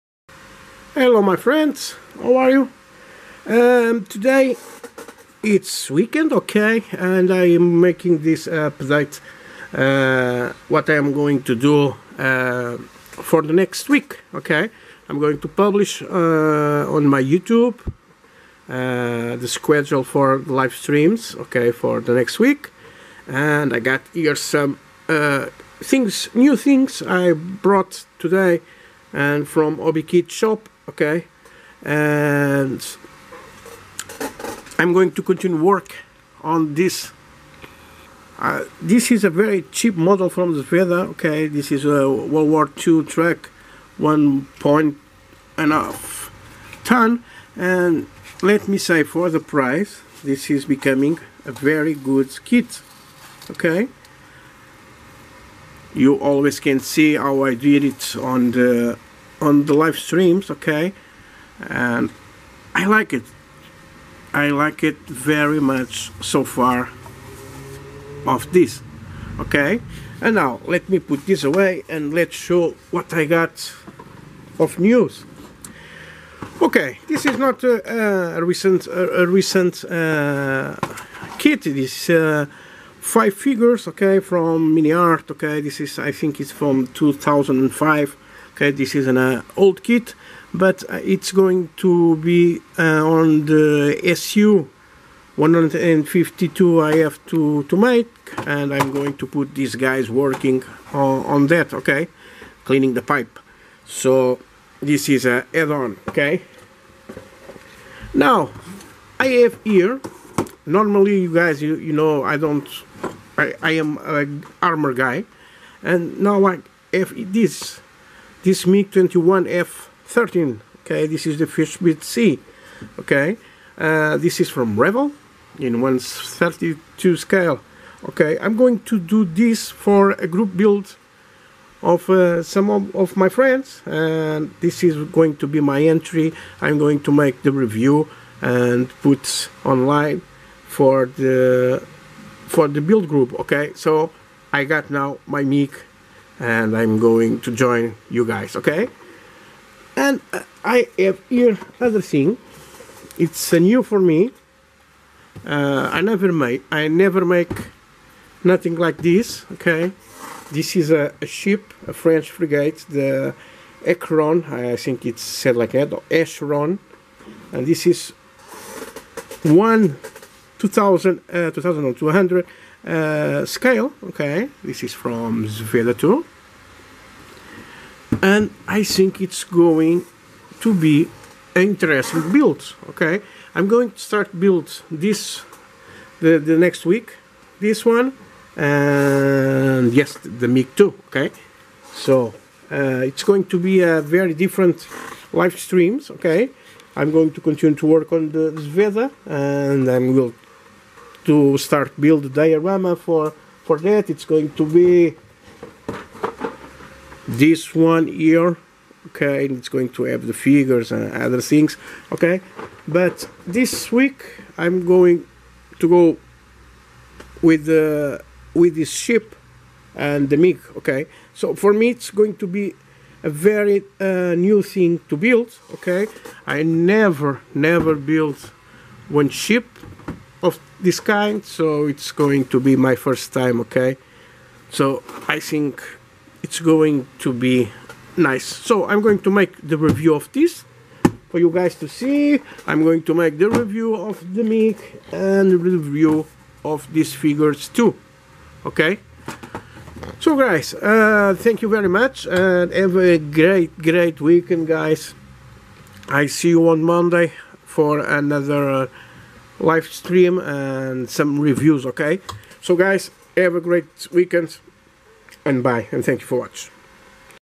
Update - New models and what will I do next week in Live Streaming
New models and what will I do next week in Live Streaming. A new adventure with Ships, a Mig Plane for the MIG Group Build and more figures from Miniart for my ISU-152. Guys have a great Weekend :)